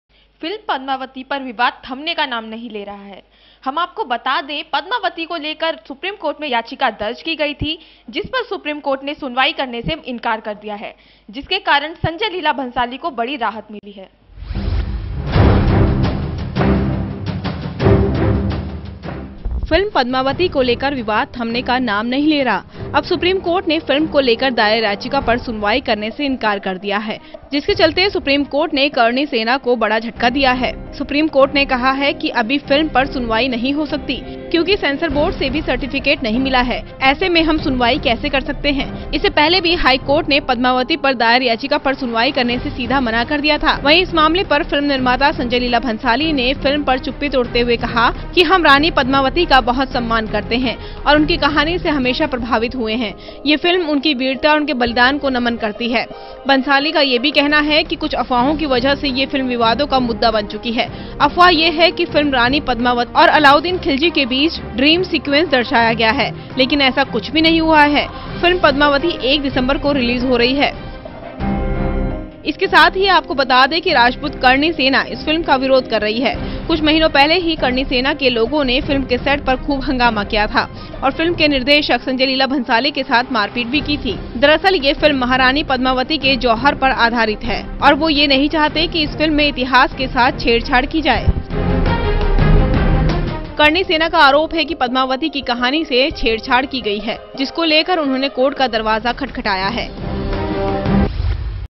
फिल्म Padmavati पर करणी सेना को सुप्रीम कोर्ट से बड़ा झटका !
फिल्म पद्मावती पर करणी सेना को सुप्रीम कोर्ट से बड़ा झटका !